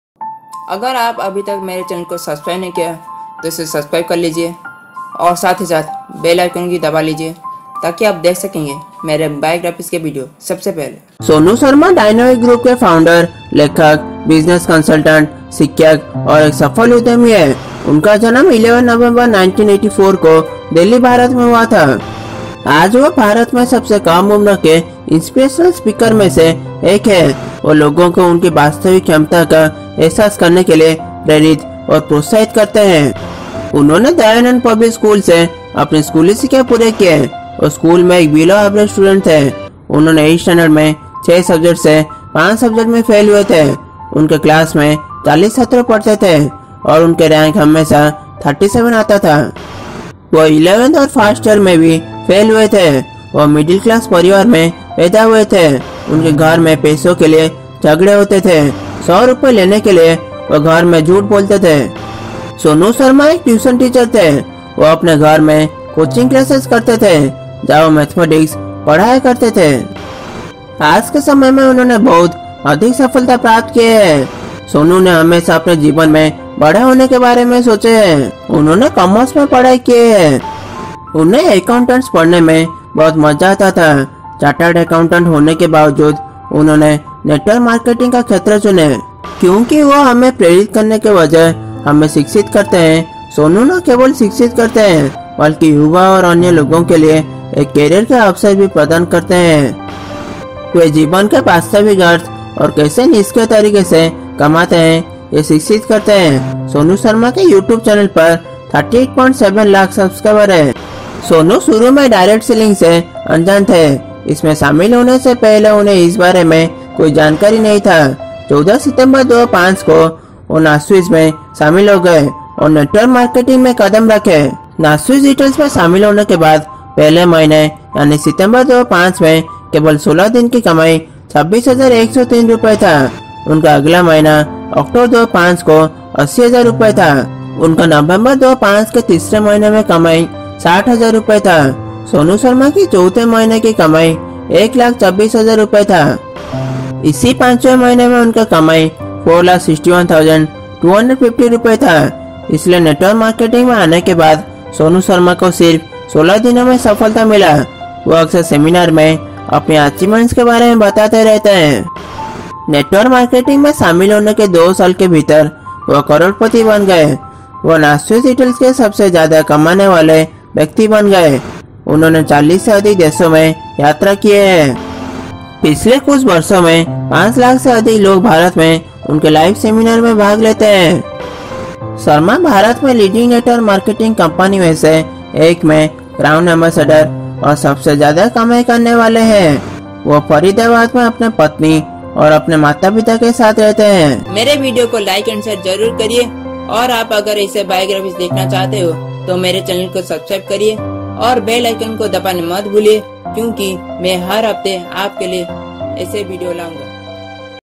Sonu Sharma Lifestyle | Biography in Hindi,Life Story,Wiki,Motivational Speech,Video,Age,Wife,Family
SonuSharma is the founder of DYNAMIC INDIA GROUP (INDIA) An Author, Educator, Business Consultant and a successful Entrepreneur, he is a much sought-after speaker. Today he is one of the Youngest Inspirational Speakers in India He inspires and encourages individuals to realize their true potential.